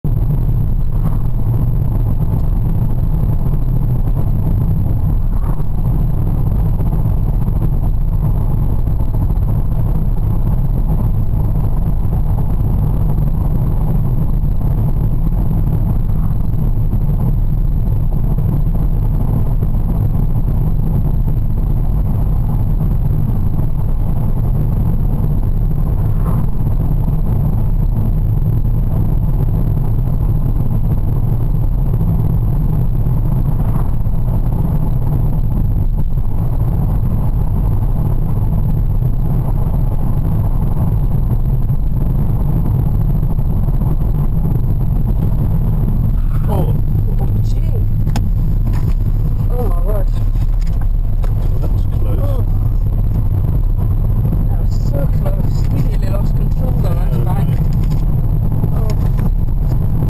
Roadhawk HD - A very near miss with a motorcycle......
This clip starts slowly but stay with it. it's only 1 minute'
I could speculate the car in front of me was becoming impatient with the car in front of him...which for some while had been driving 30/40 MPH on a 60MPH A road and did not look in his mirror before starting to overtake.
NB Separate Roadhawk video posted with speeds.location,etc